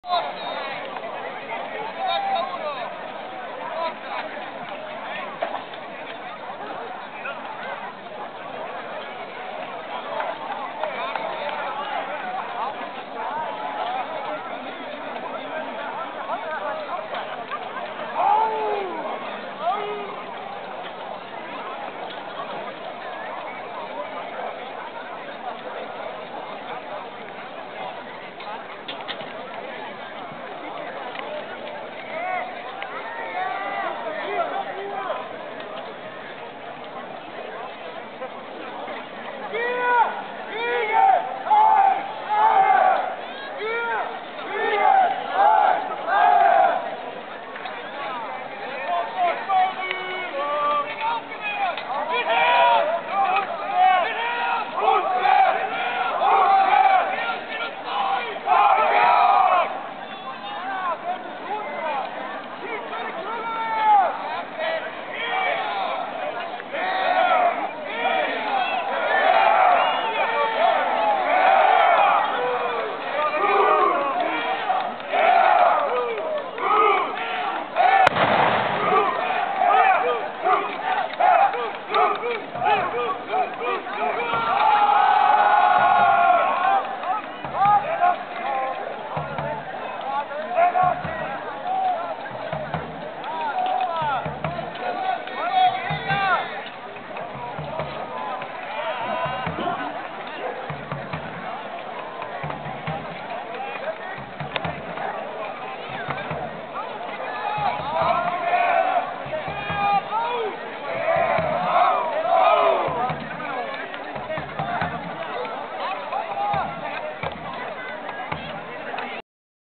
Spittal Ortenburg 2009
I giudici di campo fanno spostare l'ala Italiana al posto di quella Tedesca, dev'essere successo qualcosa di la.

The armor judges move Italian wing in place of German on the left, something must have happened there.